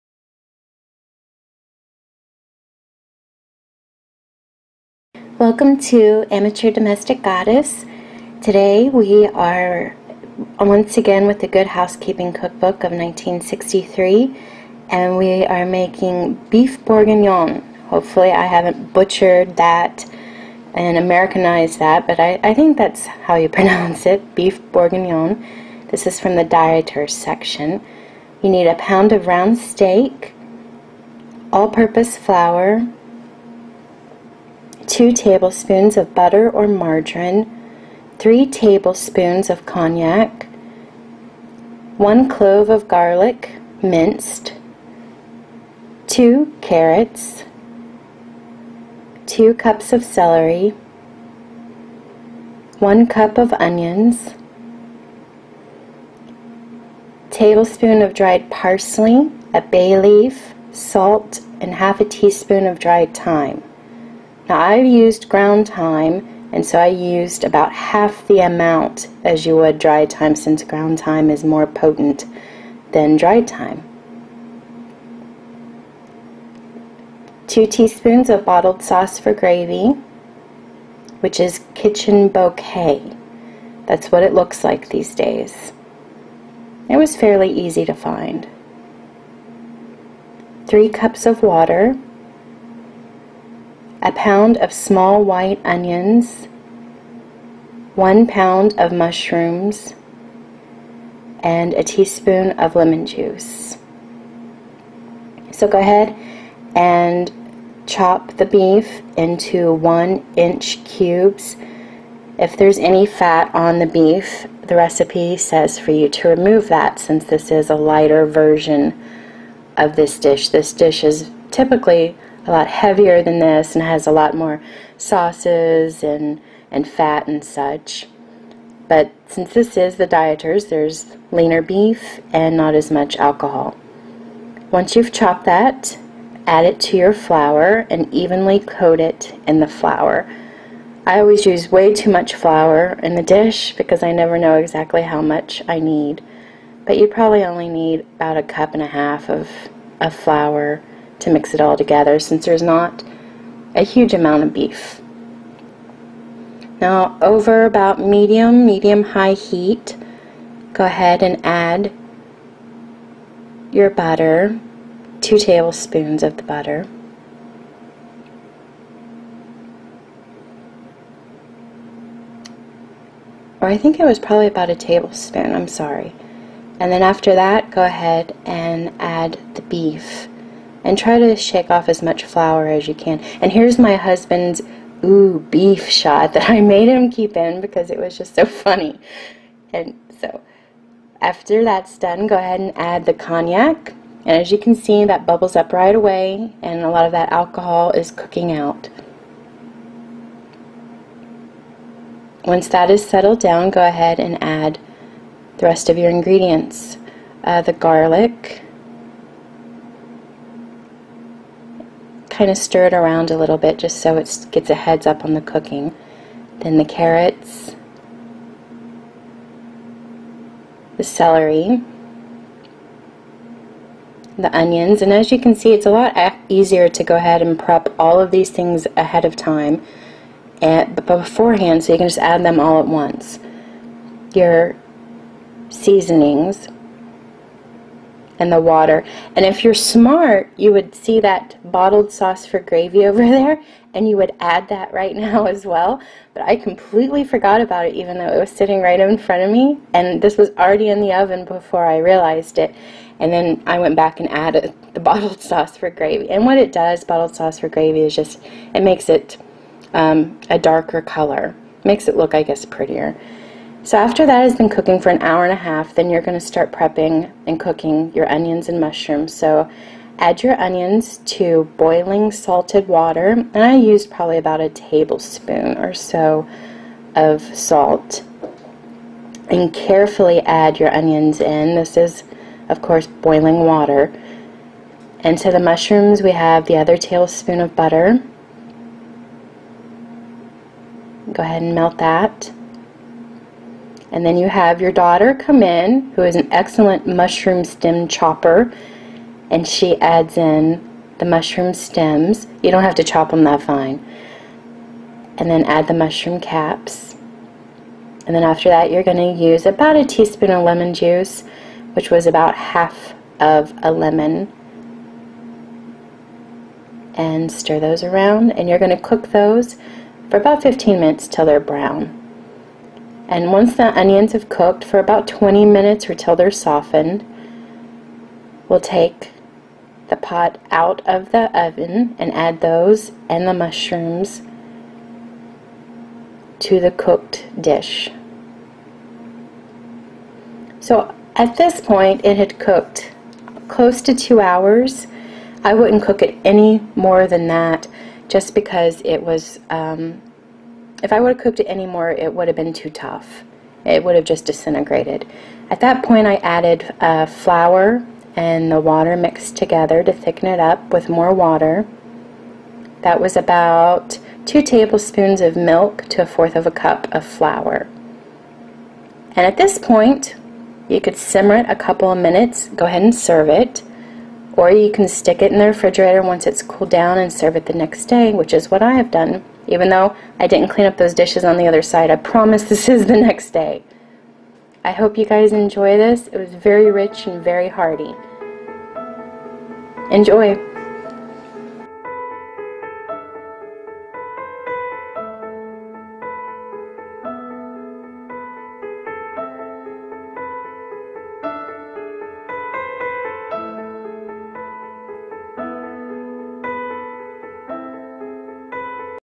Cooking - Boeuf Bourguignon II
Please note: near the end of the video I said to add, "two tablespoons of milk to a fourth of a cup of flour," when I should have said, "two tablespoons of FLOUR to a fourth of a cup of WATER." Forgive my silliness!